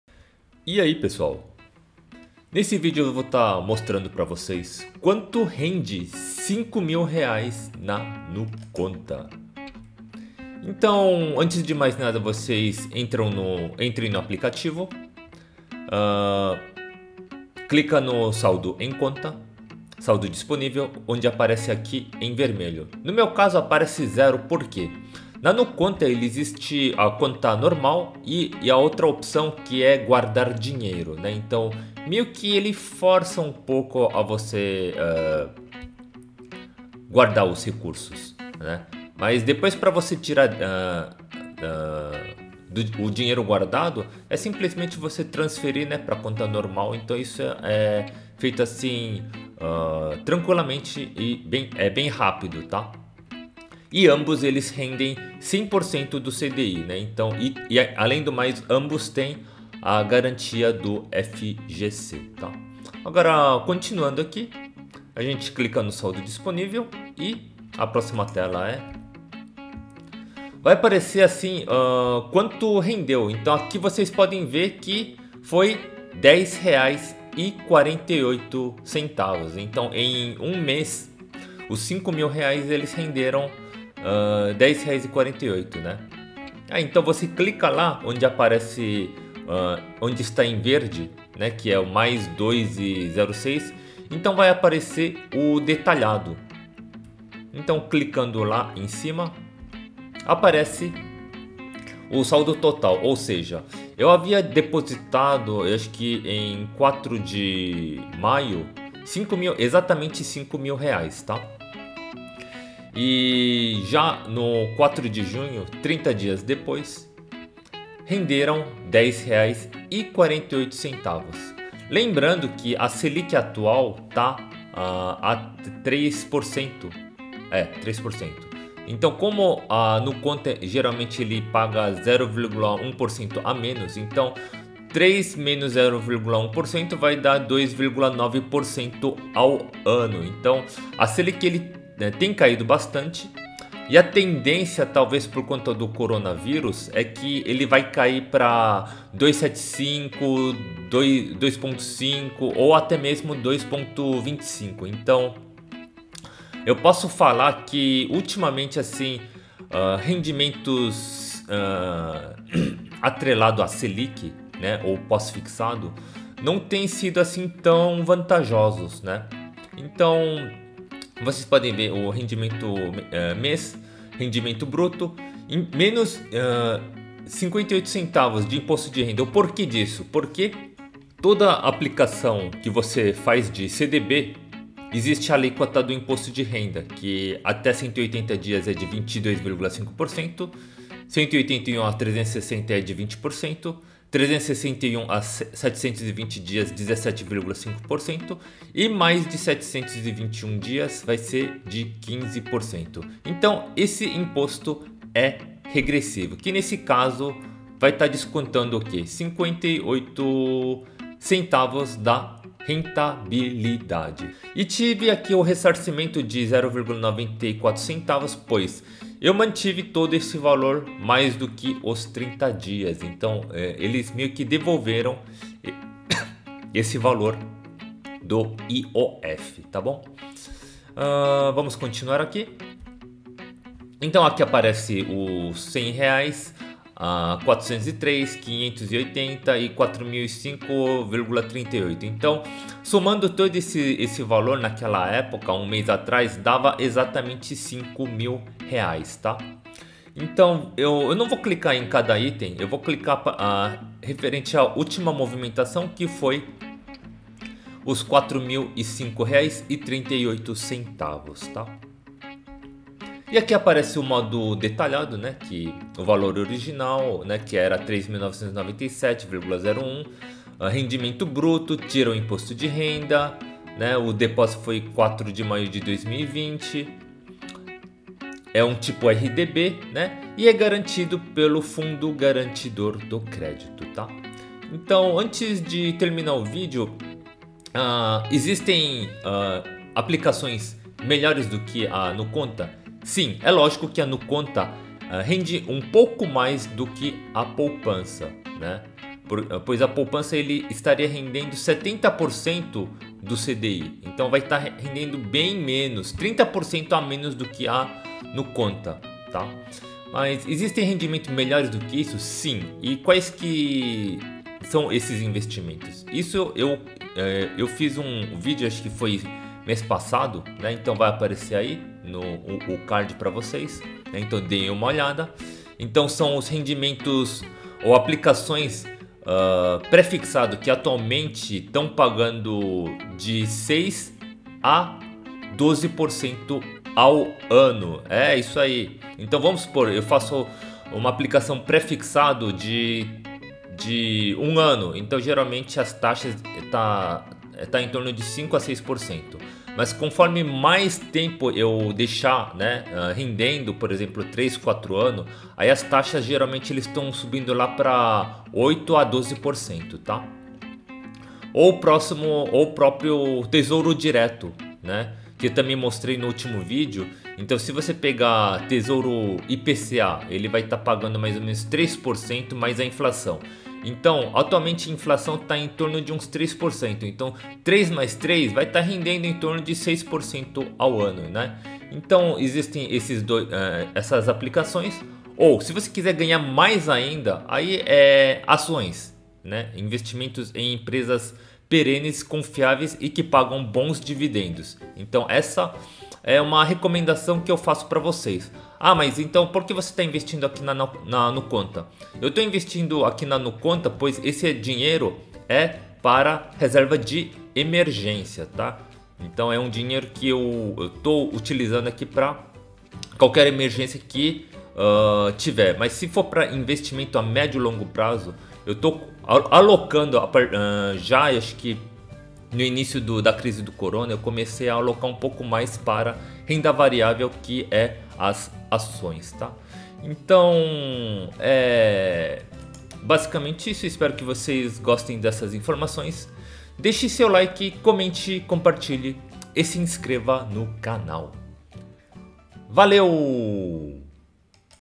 NUBANK e NUCONTA: Quanto rende R$ 5.000,00 (Cinco Mil Reais) por mês?
Um video rápido mostrando os rendimentos da Nuconta, considerando a taxa SELIC de 3,00% ao ano.

Informo que utilizo a Nuconta e seus rendimentos do RDB, apenas para a minha reserva de emergência já que ela rende um pouco mais do que a poupança.

Comparado ao Tesouro Selic, acho até um pouco melhor, pois tem uma liquidez quase que imediata, enquanto que o resgate no Tesouro demora de 1 a 2 dias úteis para cair na conta, da sua corretora.

Caso queira invertir, recomendo outras opções disponíveis de Renda Fixa na sua corretora. Ou talvez tentar aplicar na renda variável, uma vez que com a baixa da SELIC, realmente não está compensando muito investir na renda fixa.

Valeu!!